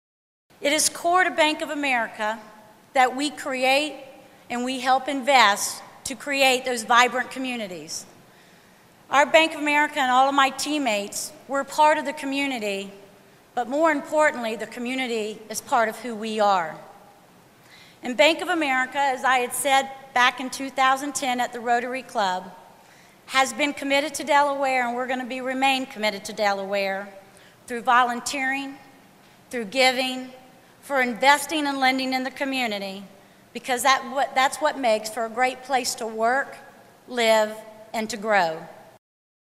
Bank of America Donation- Part 4
Bank of America increased the Delaware workforce by 500 jobs and donated their Wilmington office building for newly-created charter schools in the state. Susan Faulkner, Consumer & Small Business Products Executive, discusses the relationship between Bank of America and the community.